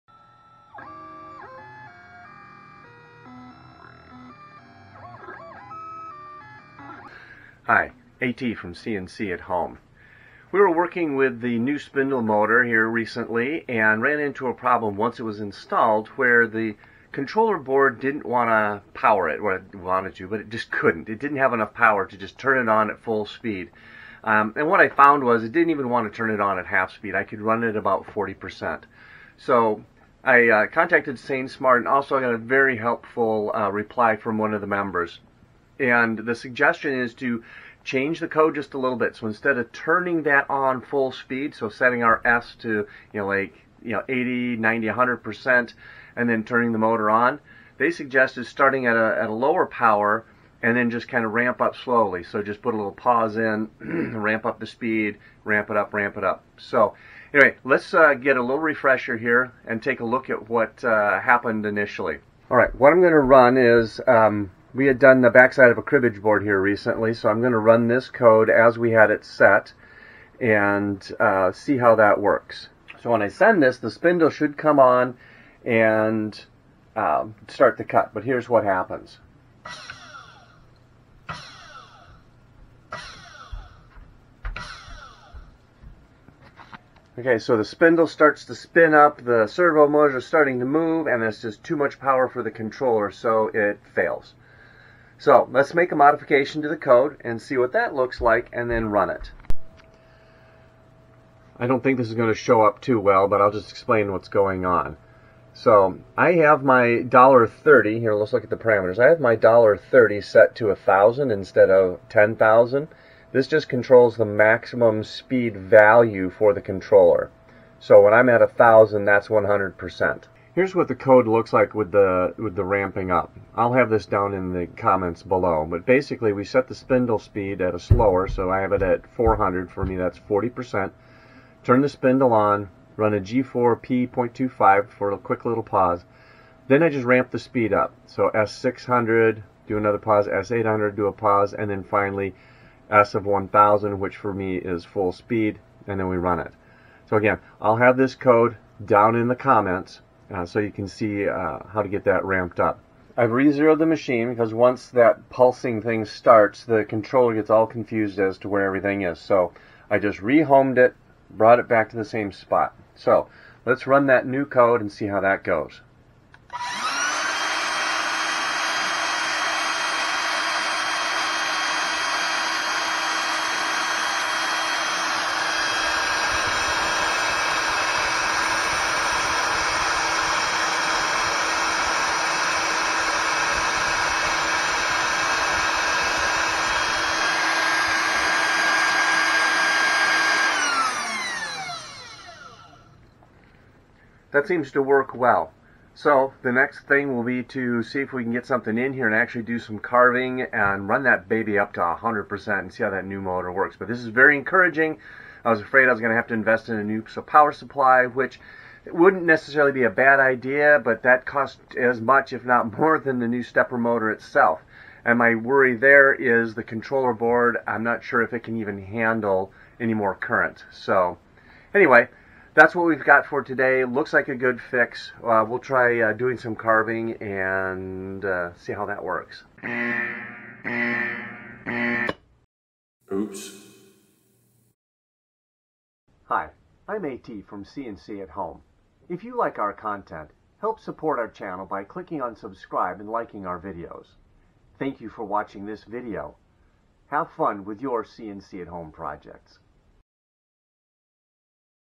SainSmart GS-775M spindle upgrade for 3018 Pro - Part II
After the initial failure of the upgraded spindle motor, there was a code solution presented by SainSmart and a CNC @ Home follower.  If the spindle speed is ramped up vs. going directly to full speed, it seems to work.  I still need to put a tool in the collet and cut something to make sure there is enough power.  But it does a pretend cut OK.

Here is the code change.  With $30=10000, replace the

S10000

M3

with this:

S4000
M3
G4 P.25
S6000
M3
G4 P.25
S8000
M3
G4 P.25
S10000
M3